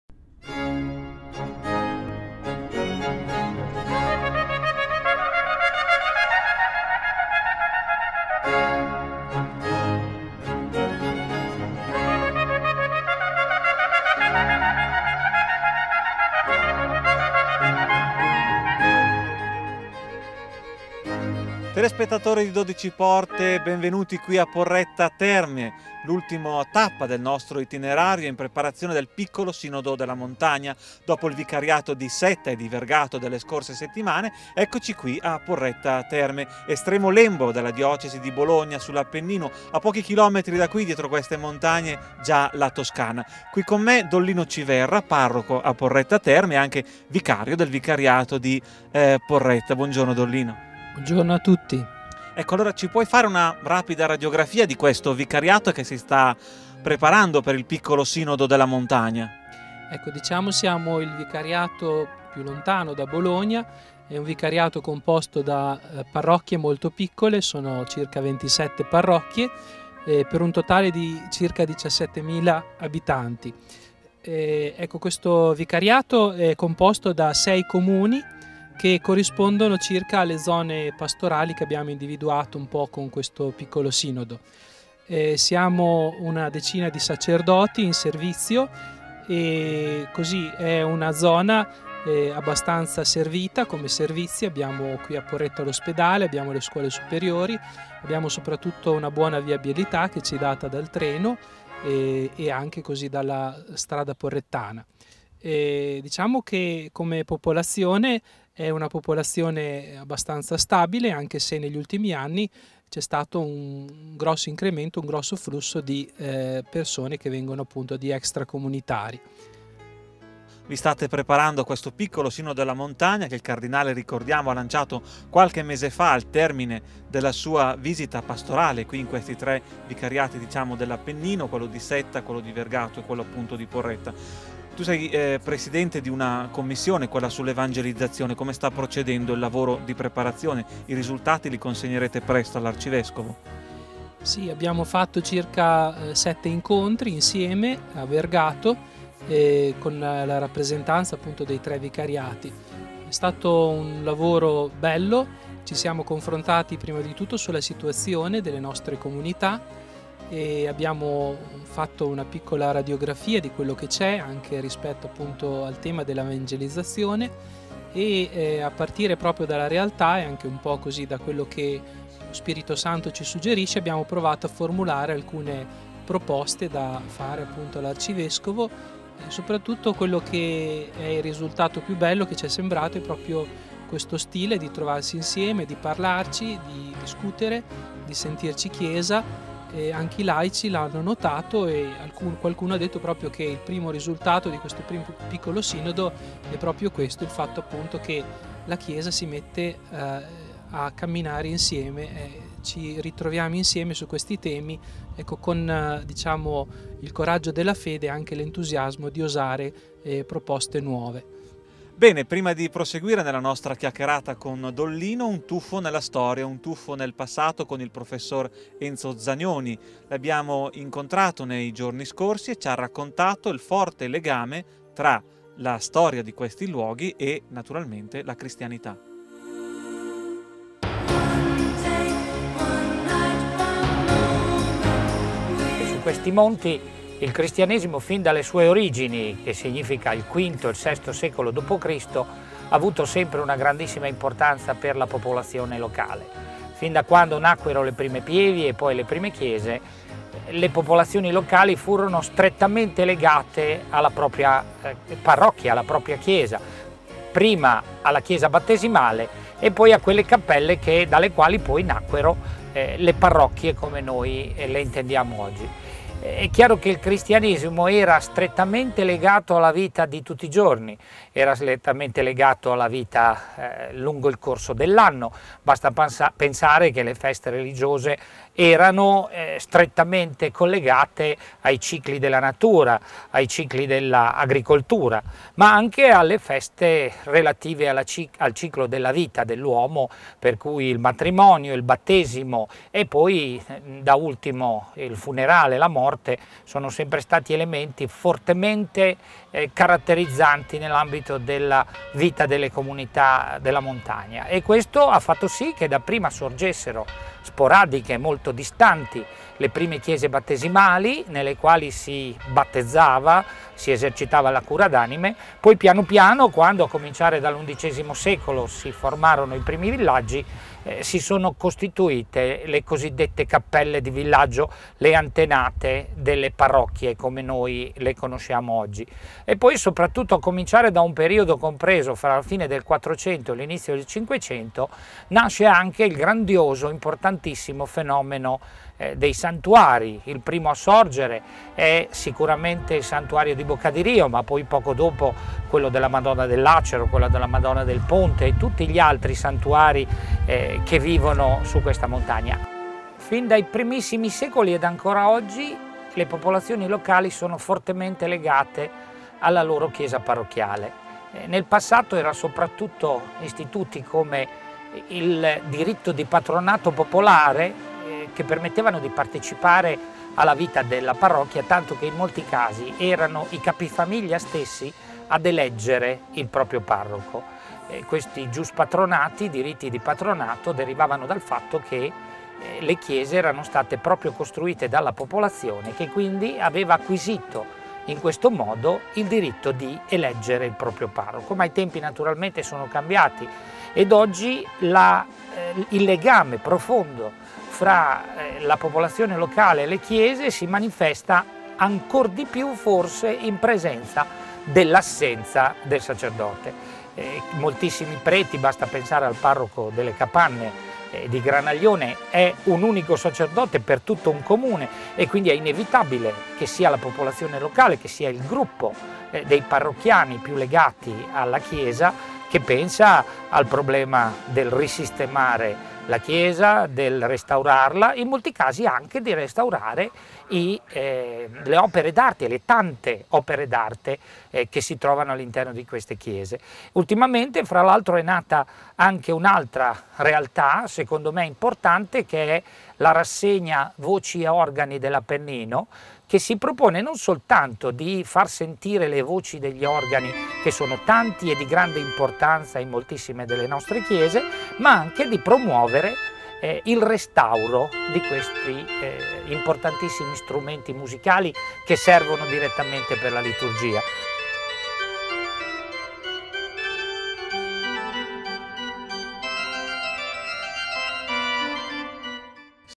Porretta per il Sinodo/1
12PORTE - 29 luglio 2010: PRIMA PARTE Andiamo dunque, come preannunciato, a Porretta Terme, dove con l'aiuto di Luciano Piacenti, Luca Tentori ha incontrato don Lino Civerra, vicario pastorale di Porretta e il prof. Renzo Zagnoni, storico della montagna bolognese. Un ampio servizio speciale che fa seguito agli altri già messi in onda e in vista del Piccolo Sinodo della Montagna.